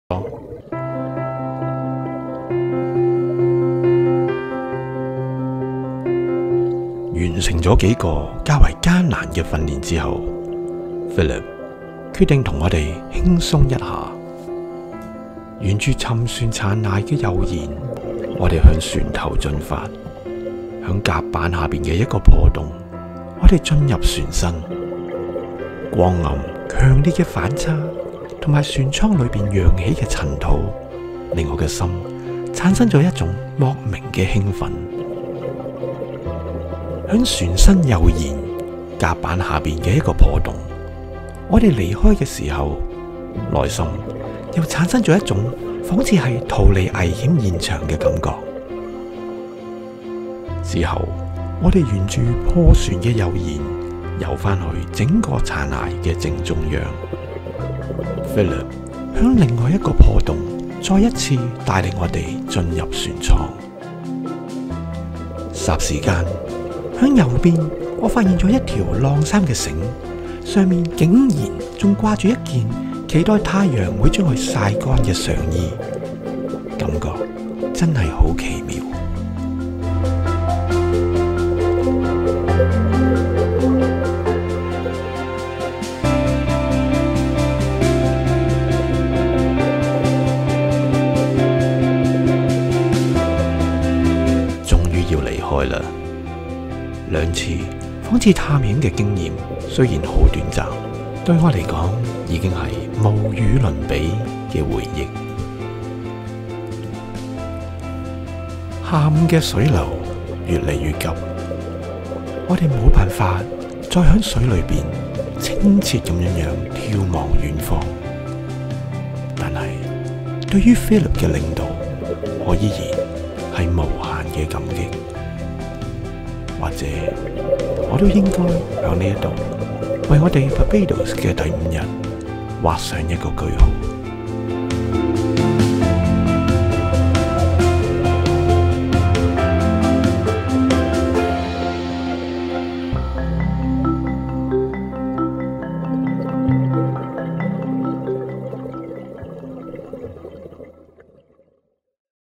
Explore a 165ft Shipwreck in Barbados North (Pamir)
Pamir is a shipwreck sank in Northern Barbados for diving purpose. Recently, a sightseeing submarine was sank 50m north from it to enlarge the diving site. Both accessible by amateur divers.

Shipwreck Pamir
Location North of Barbado (close to Maycocks Bay)
Dimension 165ft long
Depth  60 ft
Level  PADI Scuba I

Explore a 165 feet Shipwreck in Barbados after receiving my PADI scuba diving certificate. I'm glad my heart stays young even my aging. Strongly believe diving is one of the sports that serve you as an anti-aging pill. Great day.